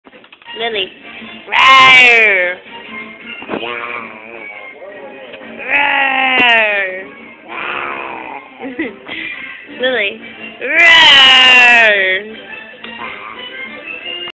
Lilypoopoo and her cute attempt at roaring!
Lily (23 mos) trying to roar like her older sister, Samantha.